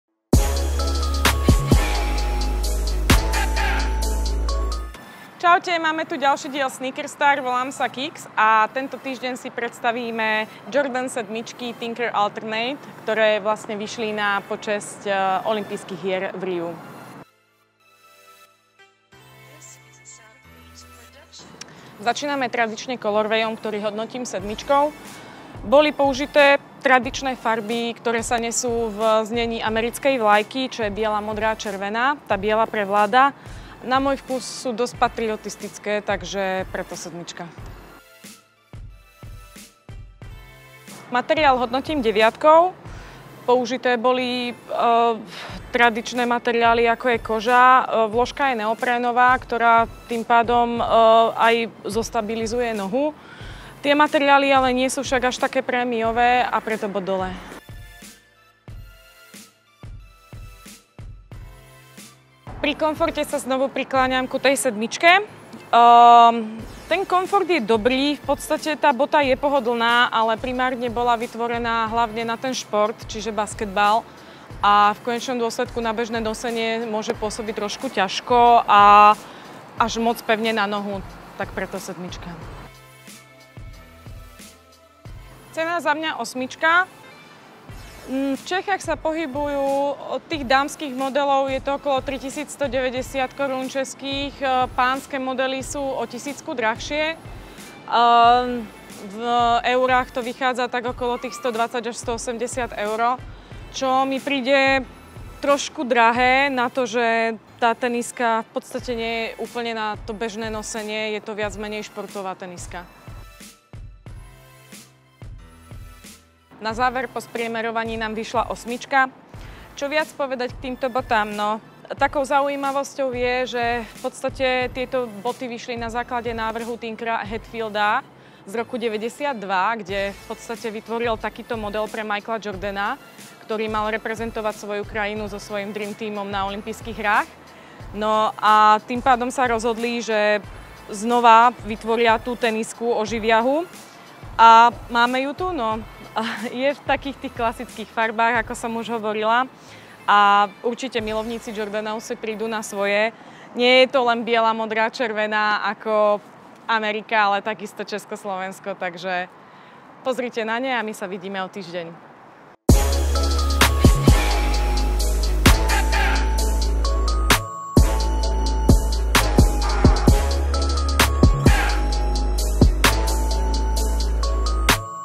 Footshop Sneakerstar #29 - Air Jordan Retro VII "Tinker Alternate"
V dnešním díle našeho pořadu Sneakerstar se po dlouhé době podíváme na tenisky od značky Jordan Brand. Kix si vybrala Retro siluetu s číslem XII, ve které Michael Jordan vyhrál olympiádu v Barceloně. Čím vším jsou tyto tenisky zajímavé? Pusť si video a dozvíš se!